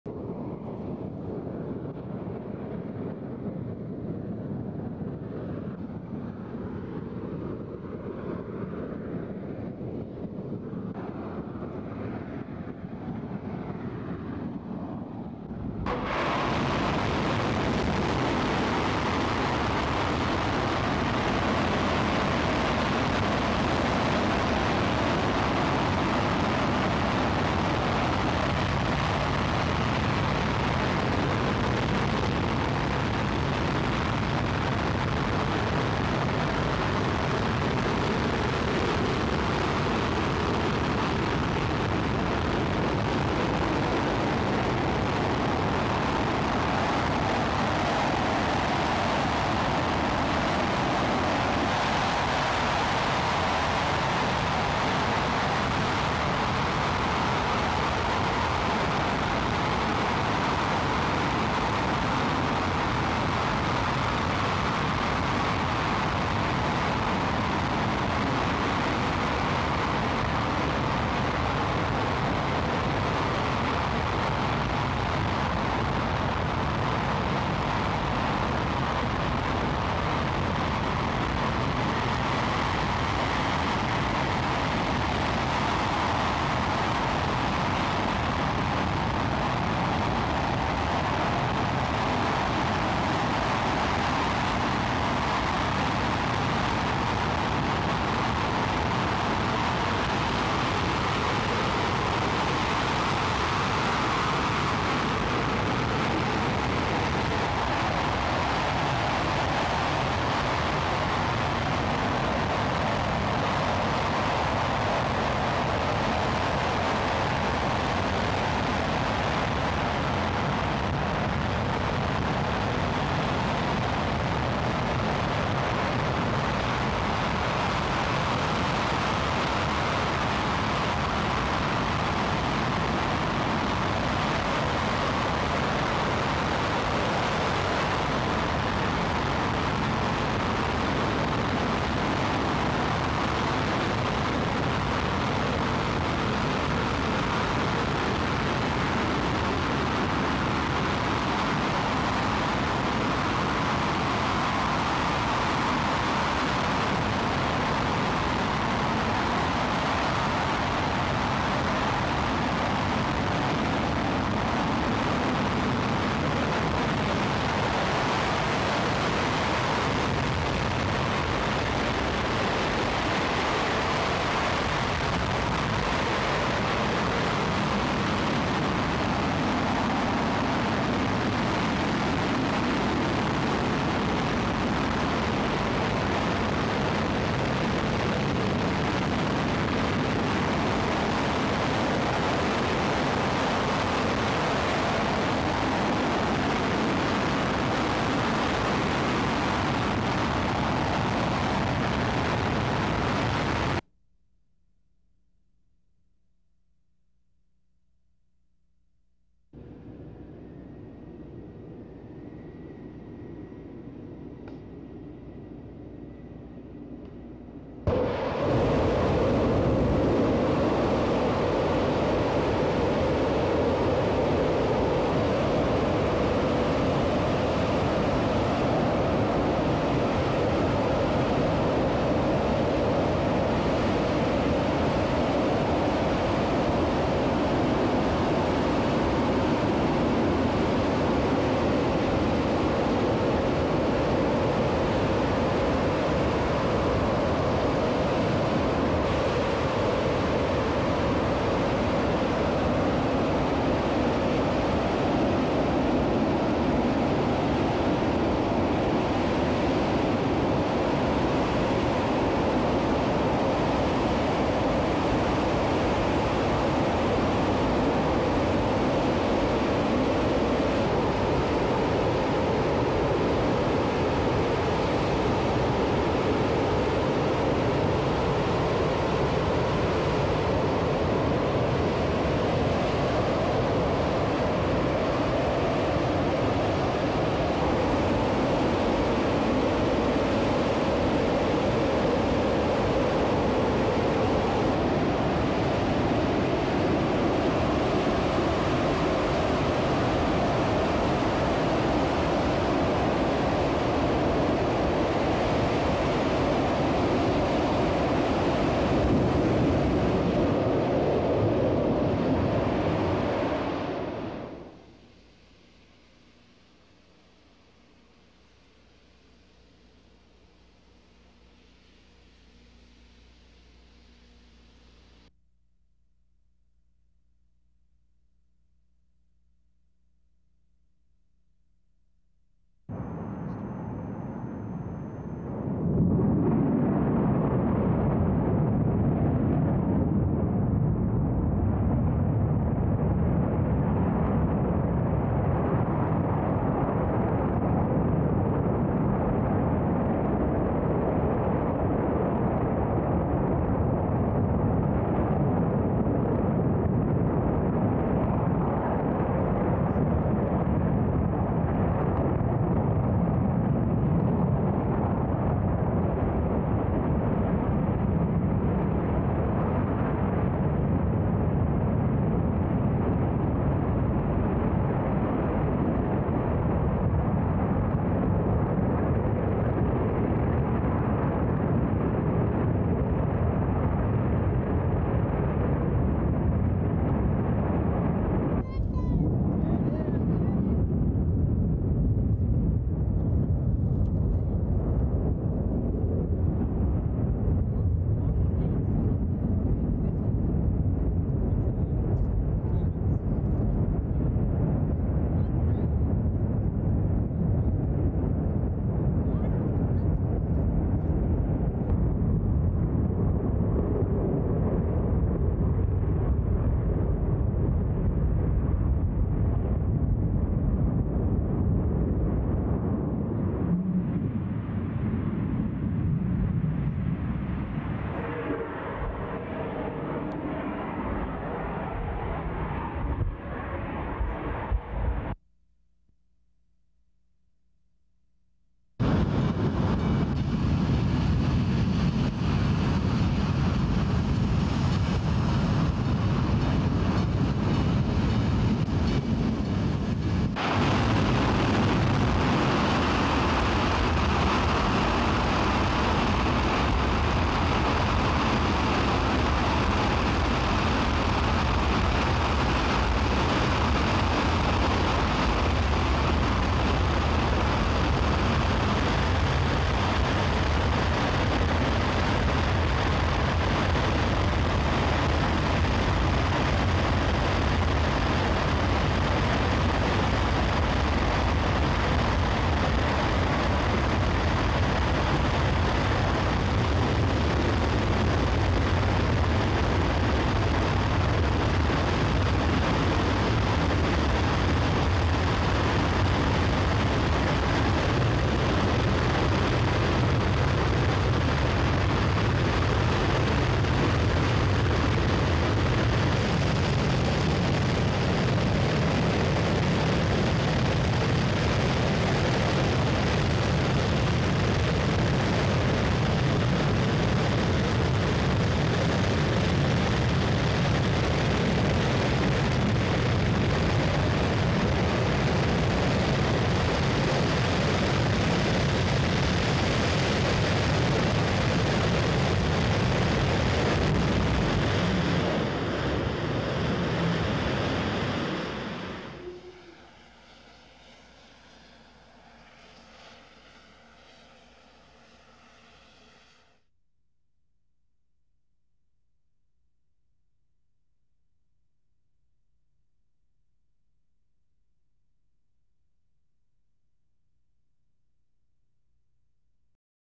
Space Shuttle Main Engine (SSME) Testing at Stennis Space Center
Different views of Space Shuttle Main Engine test firings on all three test stands including closeup of engine, day, and night firings are presented.